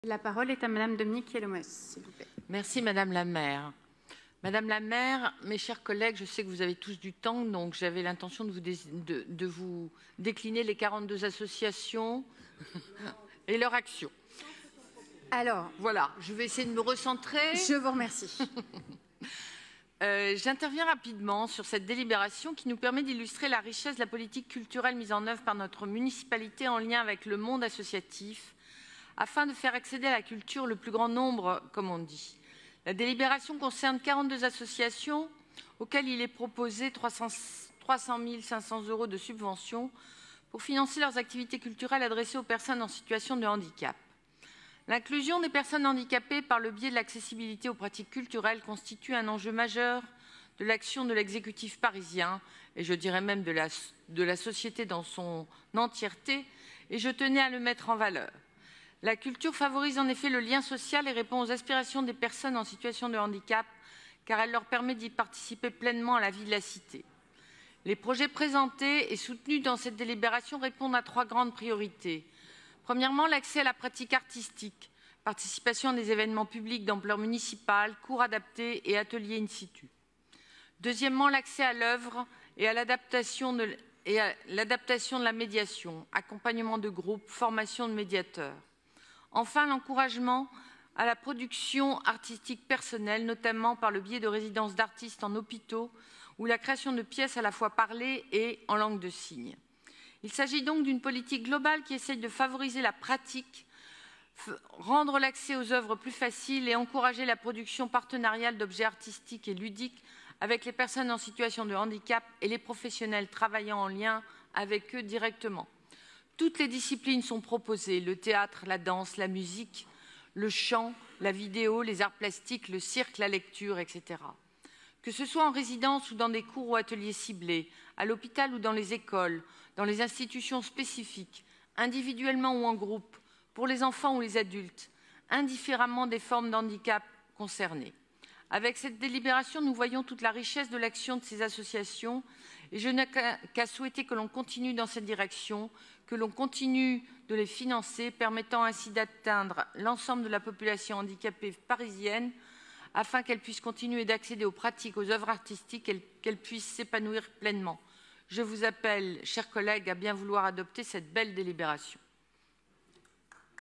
Dominique Kielemoës - DAC 364 - 42 associations au titre de Culture et Handicap 2024
2024 DAC 364 - Subventions (300.500 euros) à 42 associations au titre de Culture et Handicap 2024, et signature de 16 conventions et un avenant.